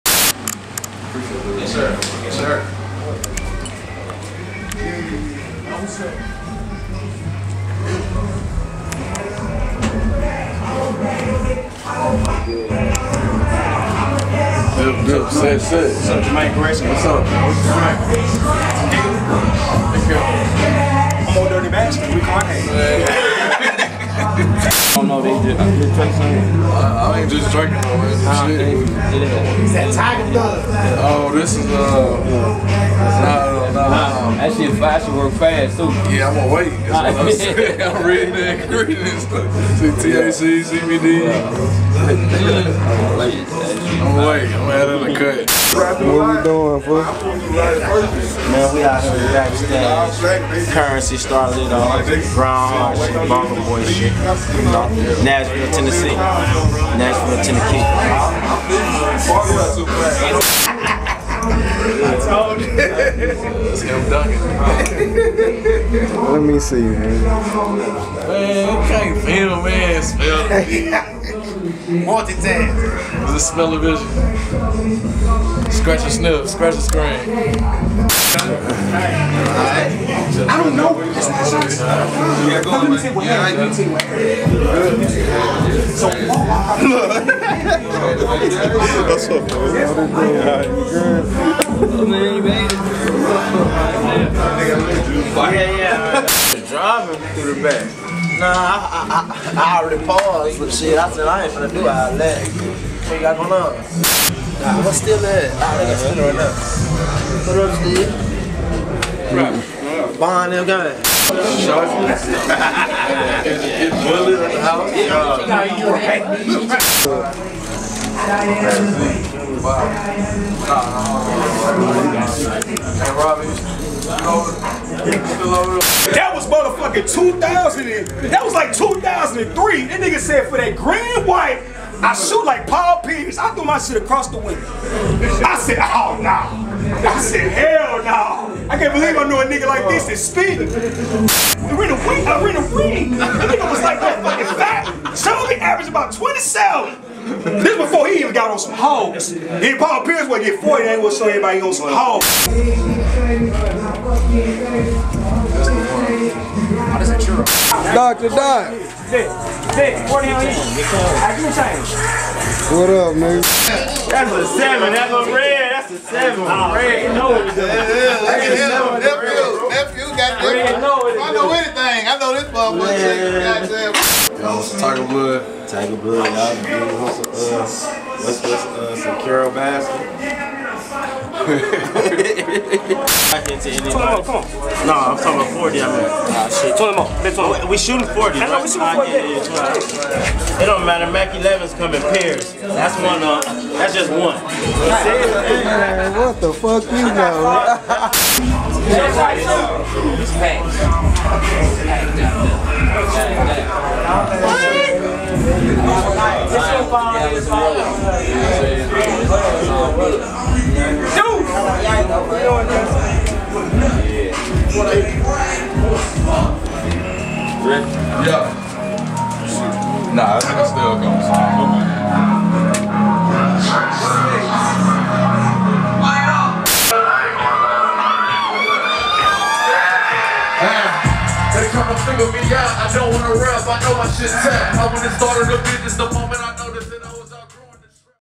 Dope Moment with Starlito - Backstage Chronicles
Backstage with Starlito and friends (Trapperman Dale, Tha Landlord, Bandplay, Red Dot) before the Curren$y & Starlito concert.